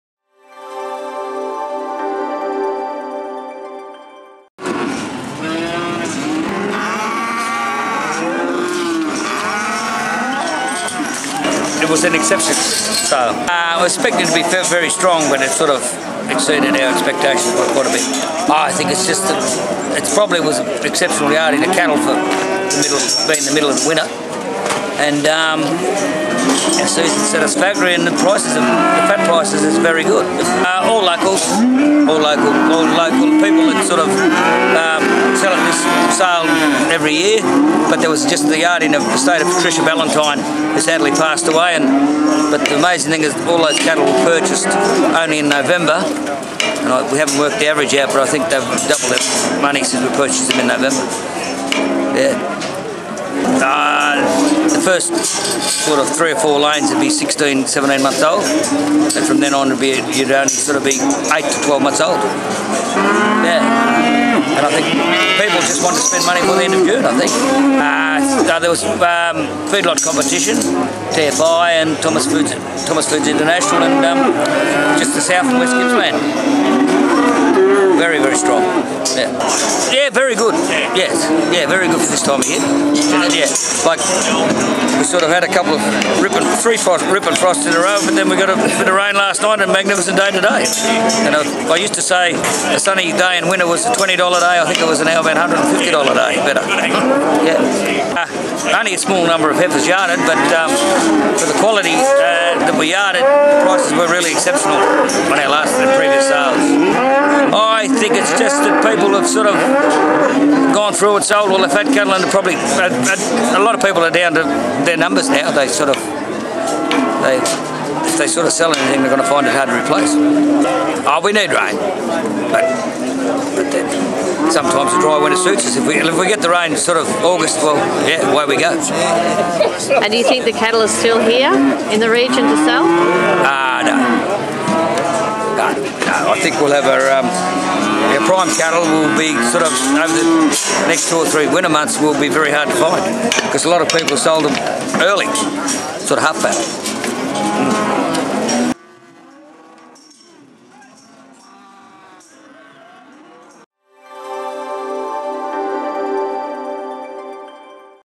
Sale store cattle sale June 5 2015 by Jeanette Severs
Store steers topped $1490 and heifers $905 in a market that saw cattle with size and weight at Sale on June 5, 2015. Jeanette Severs was there for Stock & Land.